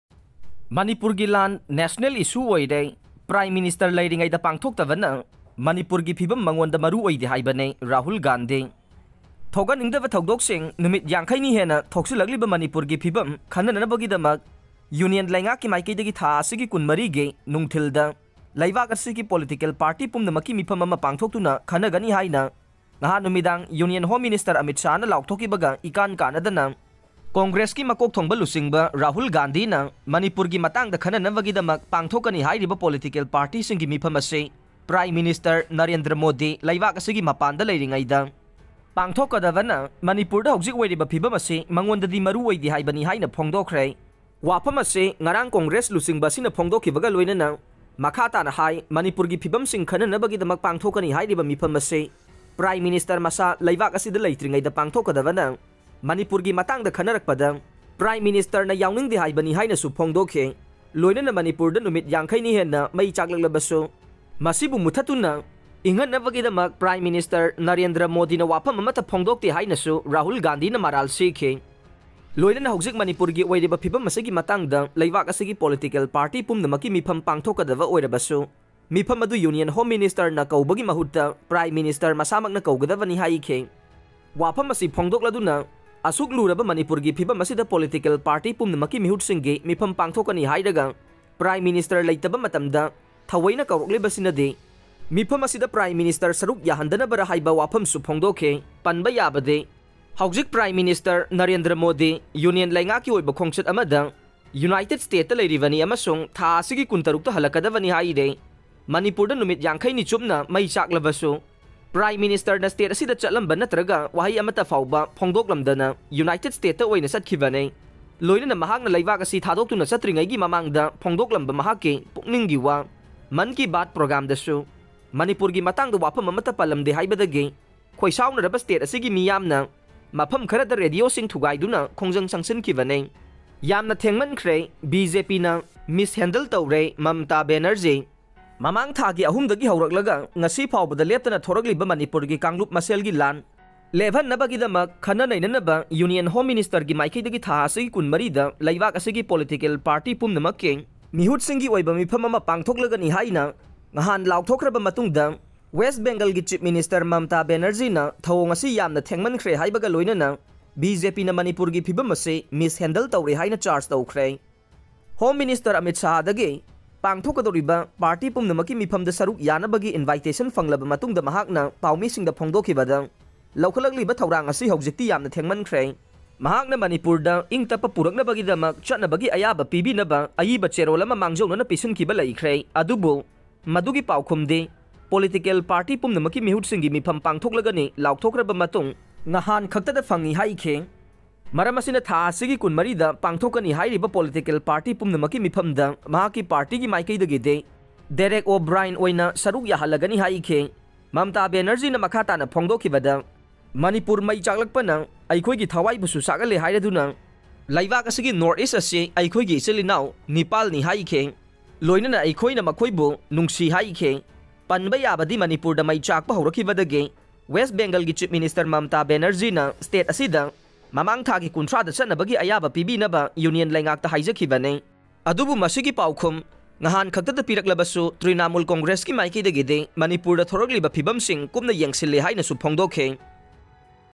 👉Manipur gi Lan National Issue Oire
👉Manipur gi Lan National Issue Oire : PM Leiringeida Pangthoktabana Manipur gi Fibam Mangonda Maru Oide Haibani : Rahul Gandhi

👉Yanna Thengmankhre, BJP na 'Mishandle' Toure : Mamta Banerjee